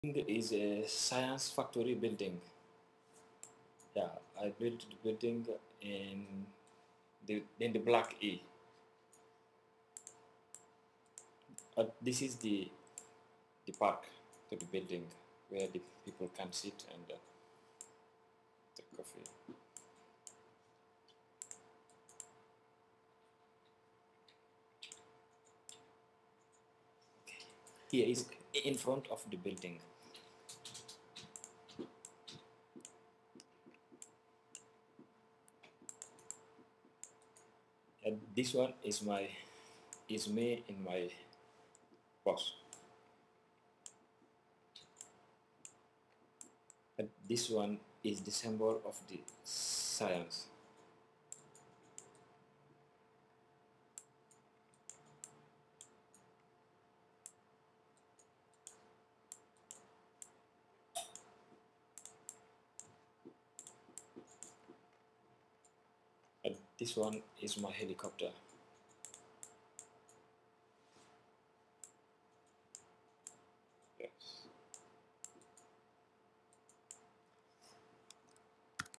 Mamadous Science Park
Mamadou explains the design for the  science park he designed using SketchUp, as a part of the 'You can get there from here.' project at the Black-E